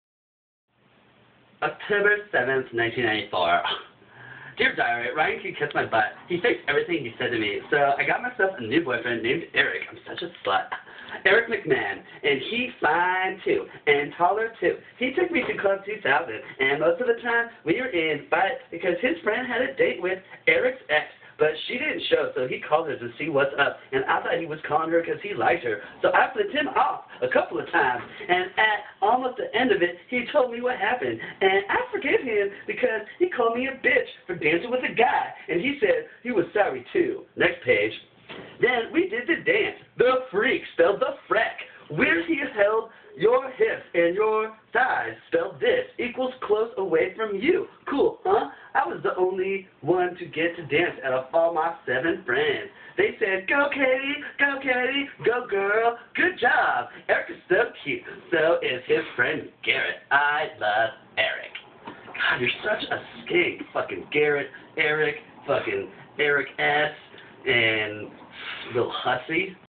Club 2000
Shirtless Boyfriend Reads from my old journal.